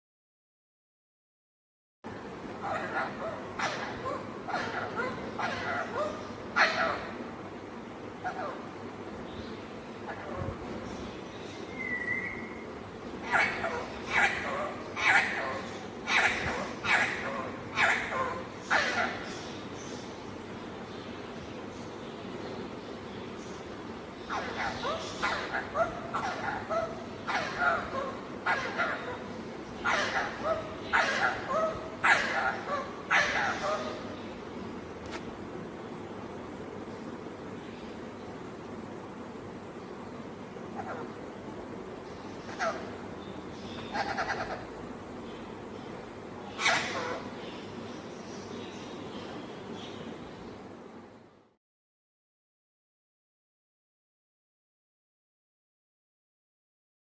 Aik prinda Allah ka zikar karty hoy
Tota Allah ka zikar krty hoy Mashallah kia baat